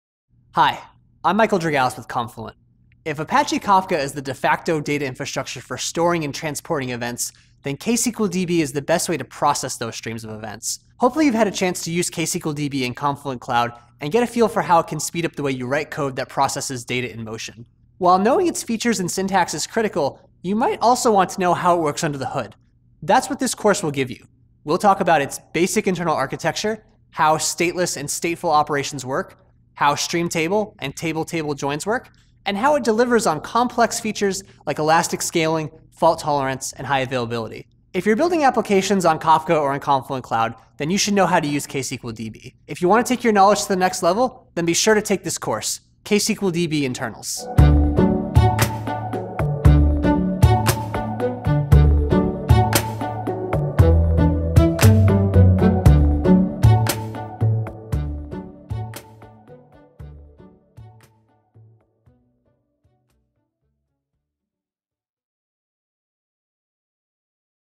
Inside ksqlDB Course Trailer | Confluent Developer
| Take your ksqlDB knowledge to the next level by joining Michael Drogalis (Principal Product Manager, Product Management, Confluent) in this course as he covers the basics of internal architecture. You will learn how stateless and stateful operations work, as well as how stream-table and table-table join work. You will also get guidance on how you can deliver on complex features such as elastic scaling, fault tolerance, and high availability. Apache Kafka® is the de facto data infrastructure for storing and transporting events, and ksqlDB is the best way to process those streams of events.